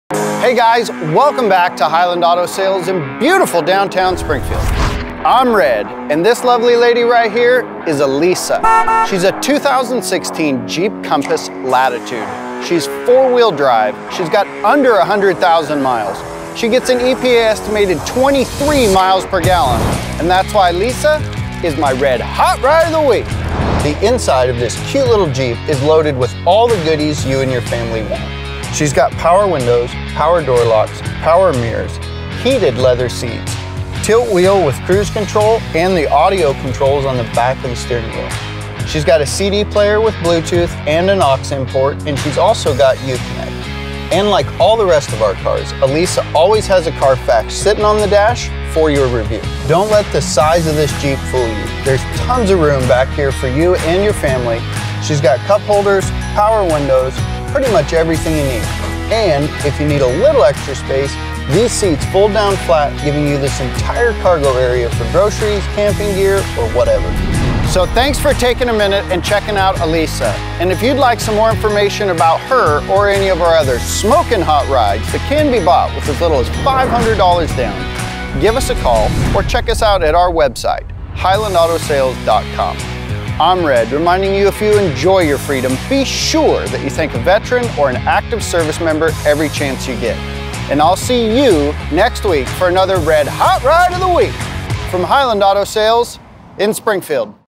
2016 Jeep Compass Latitude - Red's Hot Ride of the Week | Hyland Auto Sales
SAY HELLO TO ALESSIA! SHE IS OUR 4X4 JEEP COMPASS LATITUDE SPORT THAT HAS A CLEAN CARFAX HERE FOR YOUR REVIEW! POWERED BY A 4-CYL 2.0L SHE GETS AN EPA ESTIMATED 20-MPG IN THE CITY AND UP TO 23-MPG OUT ON THE HIGHWAY. ALESSIA IS NICELY EQUIPPED WITH AN AUTOMATIC TRANSMISSION, 4-WHEEL ABS, AIR CONDITIONING, ALARM SYSTEM, AM/FM STEREO, BLUETOOTH, CD/MP3 PLAYER, CRUISE CONTROL, FOG LIGHTS, HEATED SEATS, KEYLESS ENTRY, POWER DOOR LOCKS, POWER STEERING, POWER WINDOWS, ROOF RACK, SIRIUS XM, TILT WHEEL, AND TRACTION CONTROL.

PRICE: $11,995
COLOR: Gray
ODOMETER: 99,520
TRANSMISSION: Automatic
BODY TYPE: SUV
TITLE: Clean
CONDITION: Excellent
VIN: 1C4NJDEB2GD617102
STOCK #4663